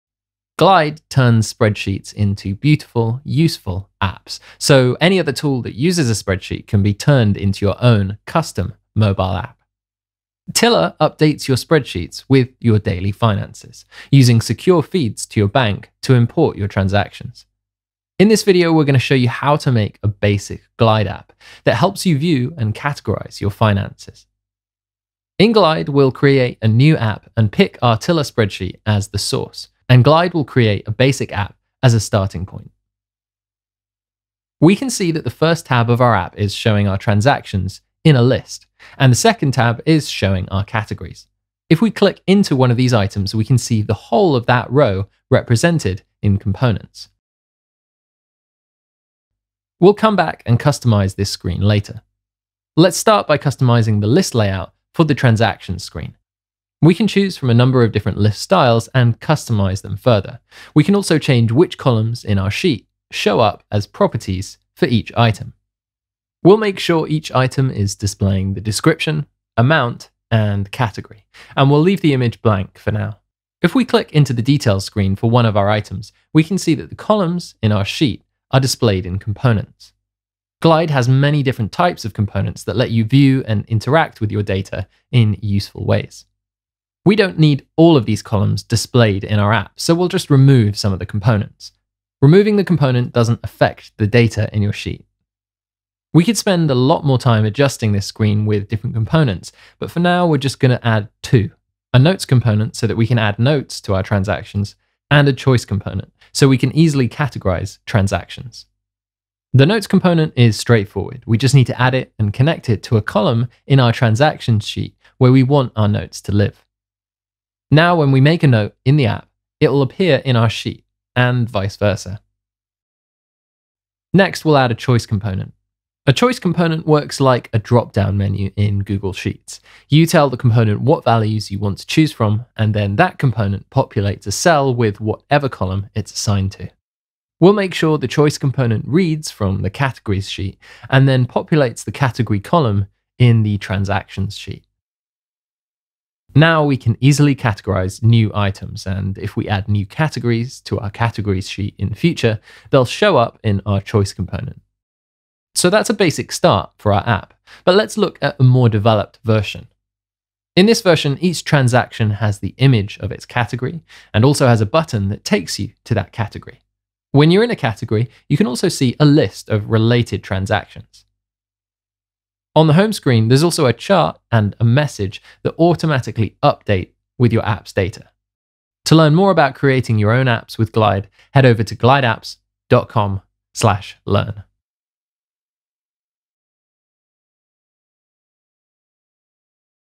Tiller Money Mobile App Tutorial: Create Custom Budget & Expense Tracker with No Code | Glide App
In this tutorial, we explore the powerful features of the Tiller Money Mobile App, a game-changing tool that transforms spreadsheets into personalized mobile applications for tracking personal budgets and expenses. With Tiller, keeping tabs on your financial transactions has never been easier!

With secure feeds, Tiller seamlessly updates your spreadsheets with your daily banking transactions. This video highlights how you can harness the capabilities of the Glide app to create your own customized application using a Tiller spreadsheet. Whether you're a tech wizard or have no coding experience whatsoever, Glide offers a no-code solution that enables you to build your very own web app effortlessly.

Discover how you can effortlessly customize the layout of your Tiller Money Mobile App, tailoring it to suit your specific needs. Remove unnecessary spreadsheet columns from the application's display without worrying about it affecting your data. With Tiller and Glide, you have the flexibility to showcase only the details that matter most to you.

Personalization is key with the Tiller Money Mobile App. Learn how to add a 'notes' component and a 'choice' component to your transaction records, making it easier than ever to categorize and organize your expenses. The 'choice' component acts like a drop-down